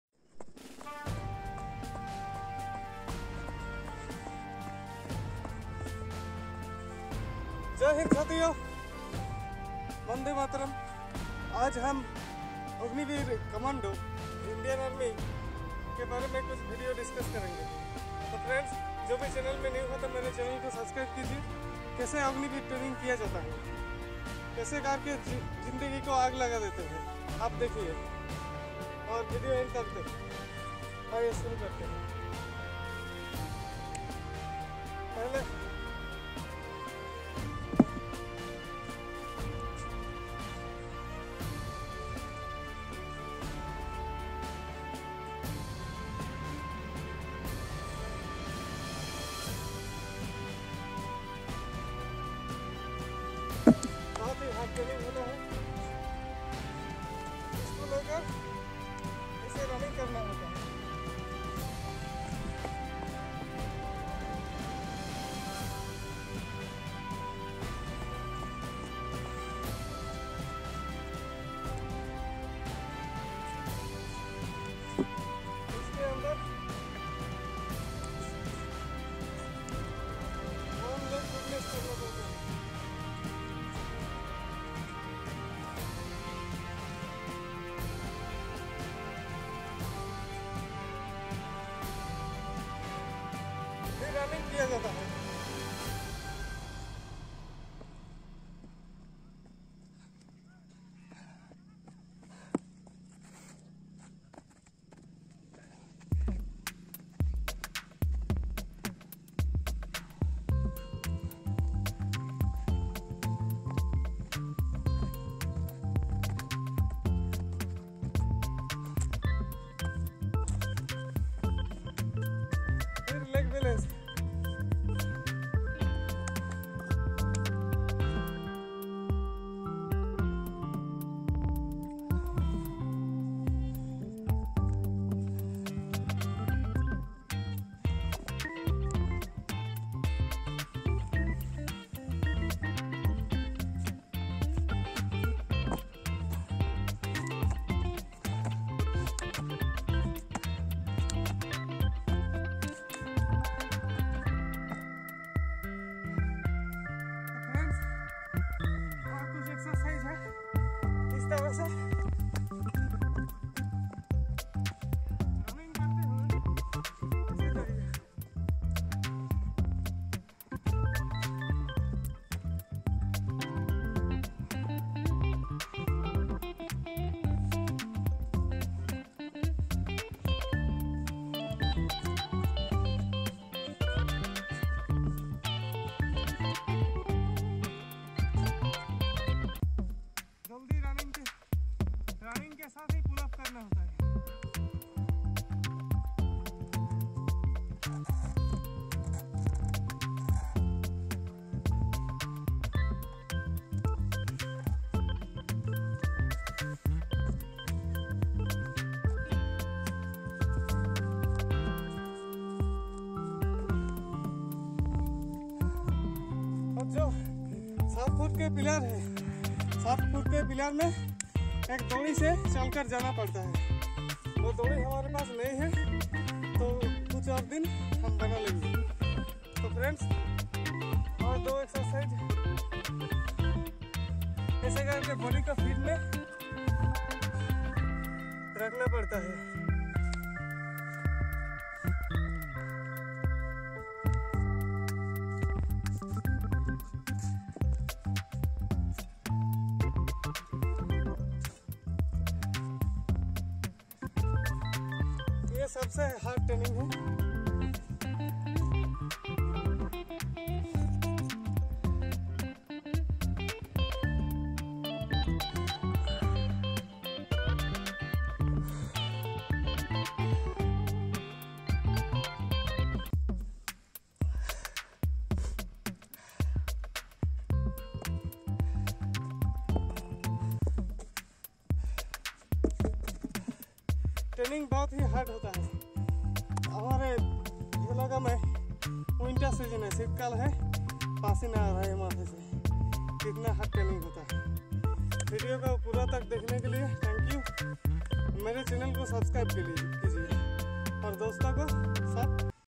1600 m running tips/ 6 मिनट वाला भी रनिंग पास करेगा 💥🛑 अग्निवीर रैली भर्ती के 4 राउंड कैसे लगाएं/
🇮🇳Tarzan Fitness Boy🇮🇳
1600 m running tips/ 6 मिनट वाला भी रनिंग पास करेगा 💥